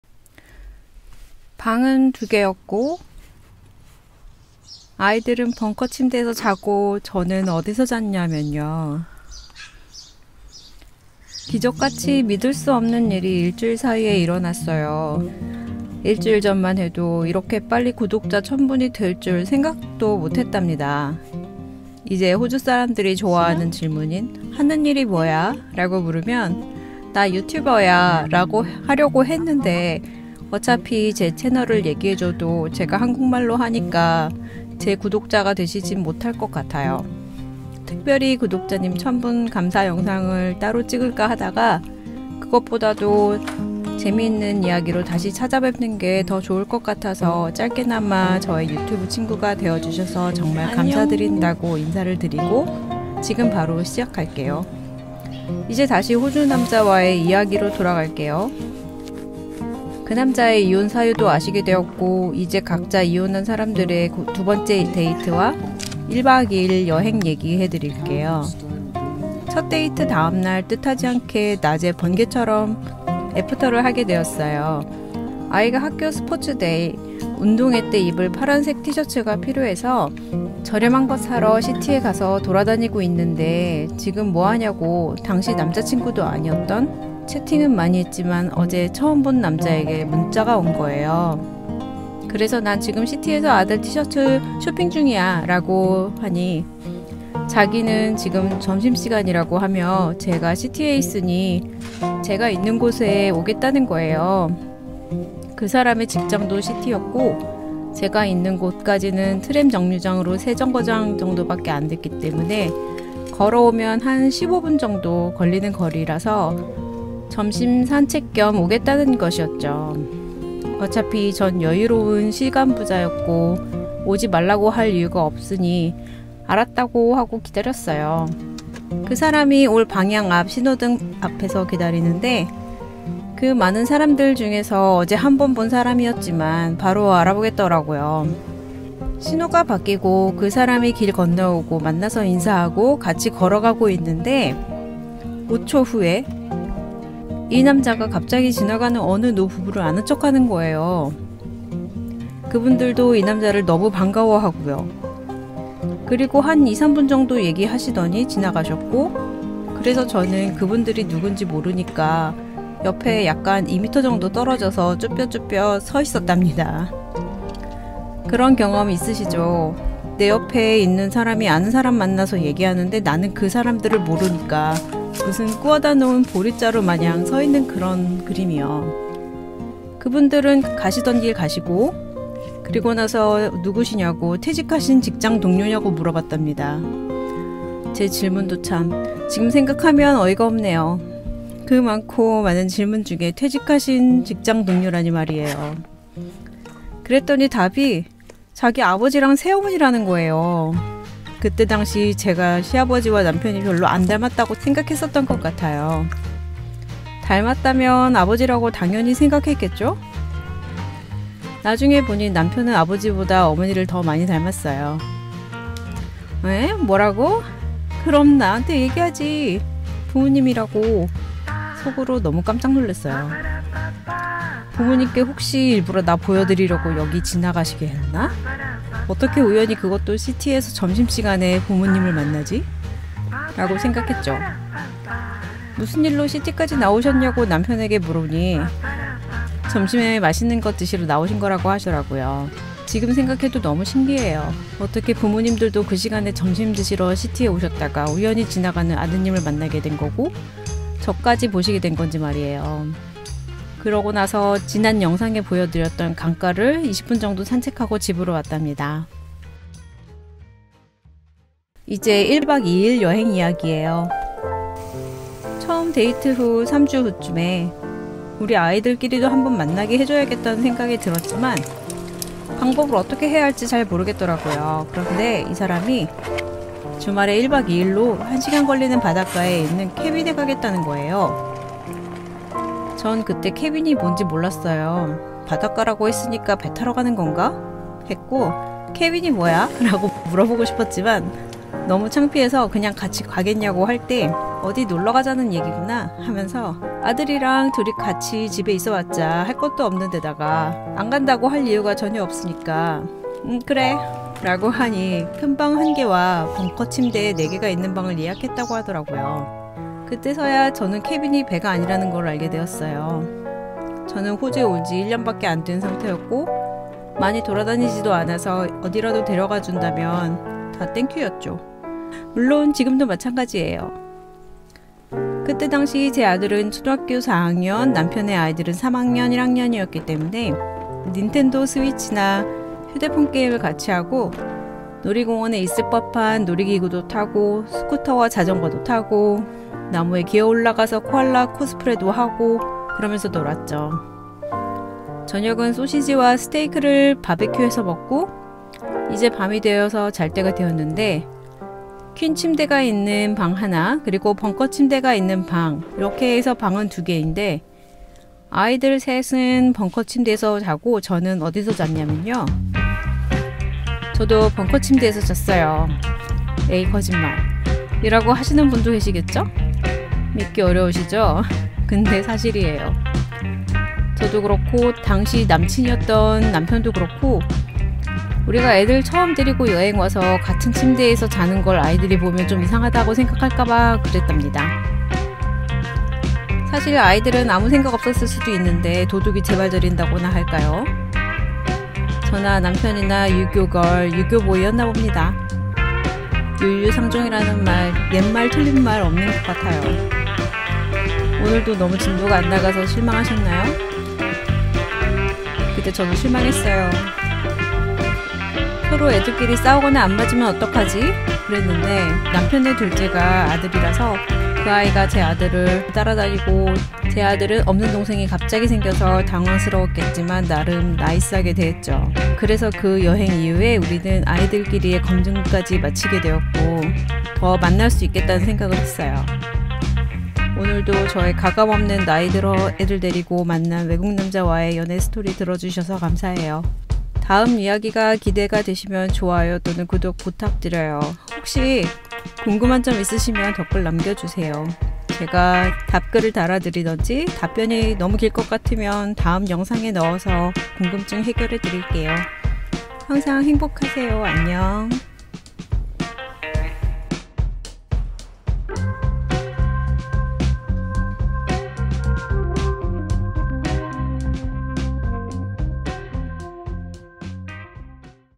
데이트 하려는데 우리 앞에 나타난 사람들은?|엄마 아빠도 모르시는 이야기|국제커플 브이로그|바게트 샌드위치, 돼지고기만두와 아이들을 위한 캥거루고기 토마토 스파게티
중년에 만난 국제커플의 이야기
구독자 1000명 감사 영상 겸 두번째 데이트와 첫 1박2일 여행 이야기입니다.
남편이 직접 만드는 바게트 샌드위치
제가 먹고 싶어서 오랜만에 만든 돼지고기 만두와 소고기대용으로 캥거루고기로 만든 토마토스파게티 만들어먹는 국제커플의 일상 브이로그

재밌게 봐주시면 감사해요! 👍😊🥰

감사합니다.